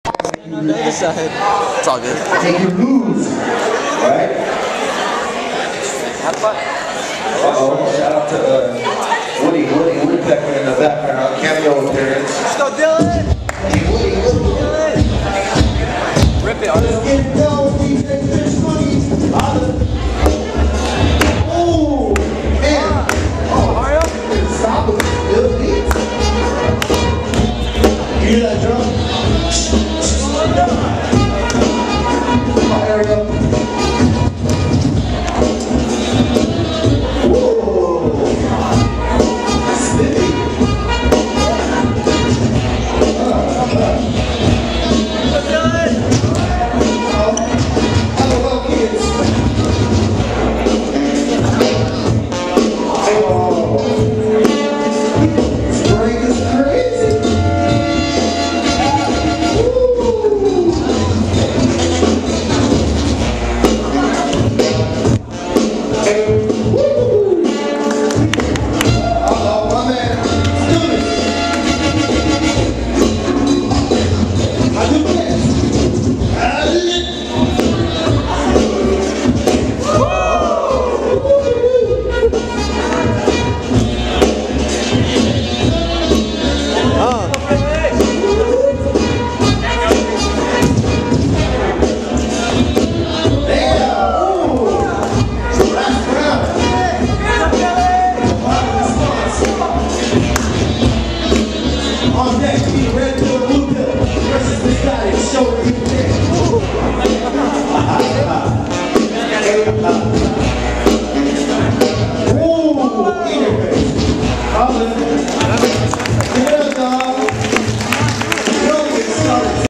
HadouKENT vs. LOLKidz | REALITY CHECK
REALITY CHECK
Bboy Battle | Preliminary

Seattle, Wa
2 on 2 Bboy Battle
1 on 1 All Styles Battle

ShoryuKENT: Hello I'm Alan, Ariel